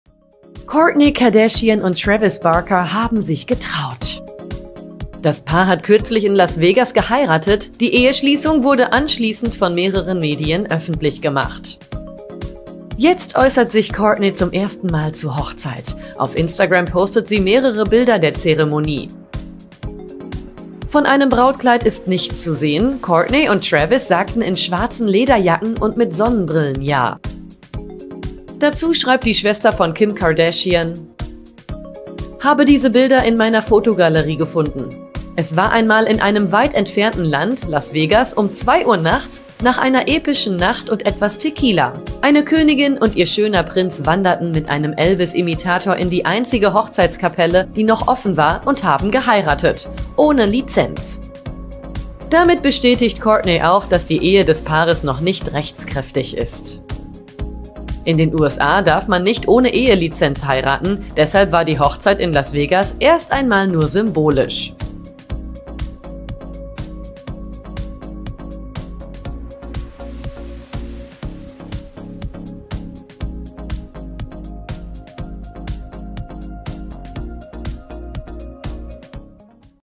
„Epische Nacht“: Kourtney Kardashian bestätigt Hochzeitsgerüchte! | It's in TV
Kourtney Kardashian und Travis Barker haben sich getraut. Das Paar hat kürzlich in Las Vegas geheiratet, die Eheschließung wurde anschließend von mehreren Medien öffentlich gemacht.